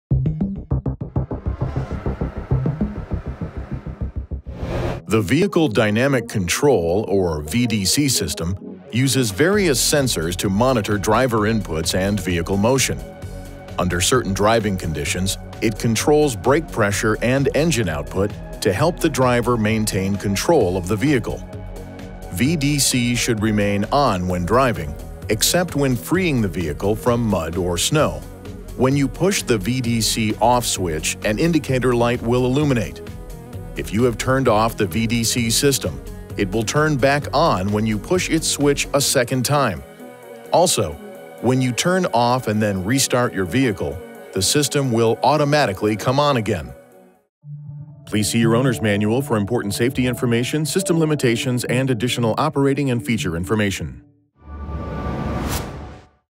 2020 INFINITI QX80 - Vehicle Dynamic Control (VDC)
The Vehicle Dynamic Control or (VDC) system uses various sensors to monitor driver inputs and vehicle motion. Under certain driving conditions, it controls brake pressure and engine output to help the driver maintain control of the vehicle. VDC should remain ON when driving, except when freeing the vehicle from mud or snow.

When you push the VDC OFF switch an indicator light will illuminate. If you have turned OFF the VDC system, it will turn back ON when you push its switch a second time. Also, when you turn OFF and then restart your vehicle, the system will automatically come on again.